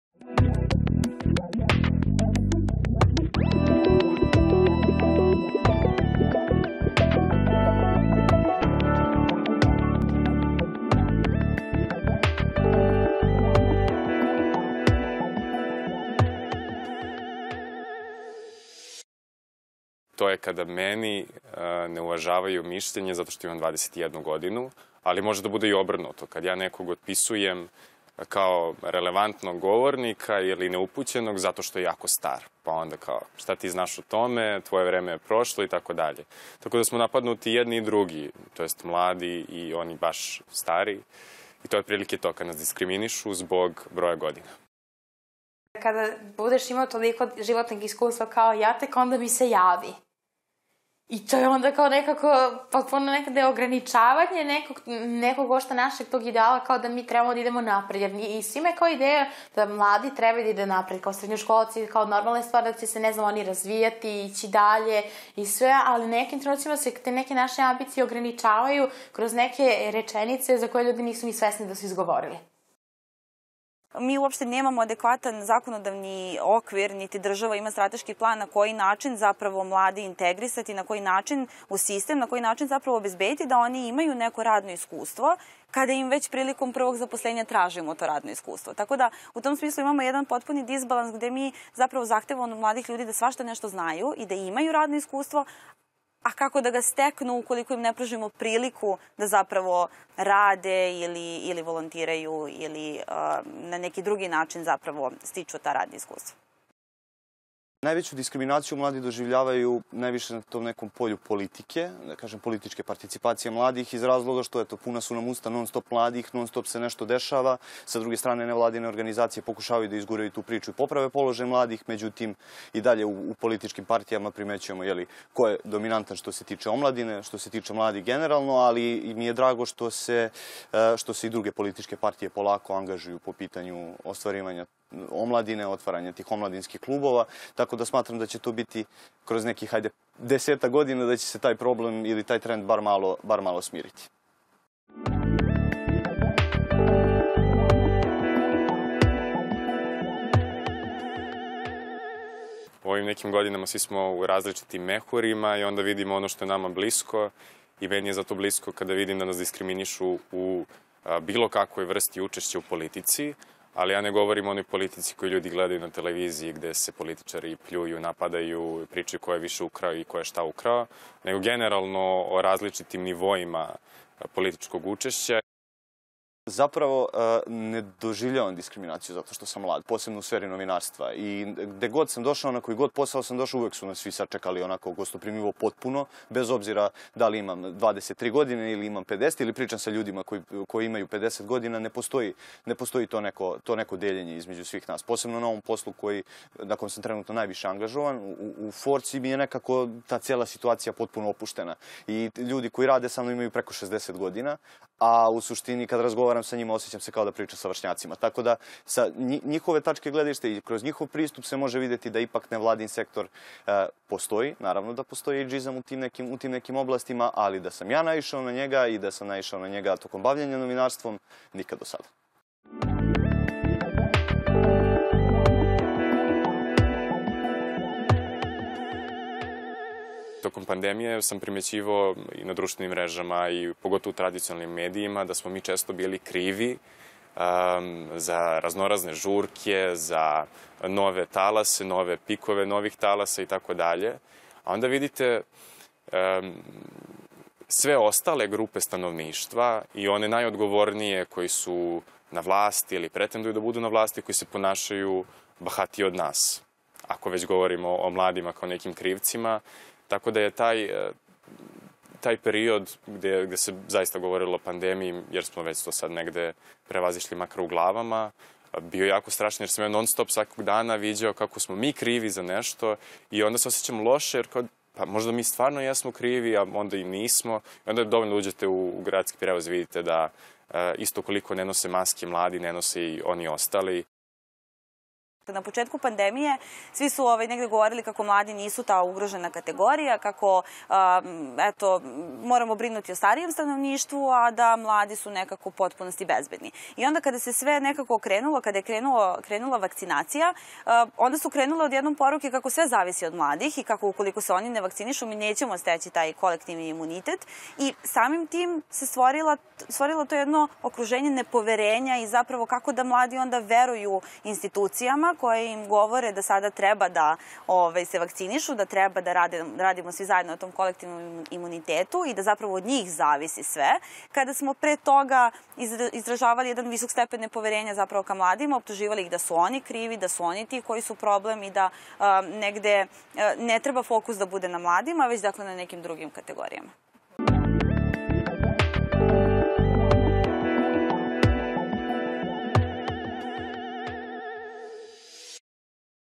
Mladi okruženi nepoverenjem
Mladi su tokom pandemije često bili optuživani zbog organizovanja žurki, a potom i novih talasa i pikova zaraze, izjavio je FoNetu istraživač Lazar Simić, ali je ukazao da su ostale grupe stanovništva, pa i ljudi na vlasti ili oni koji pretenduju da budu na vlasti, bili mnogo bahatiji od mladih.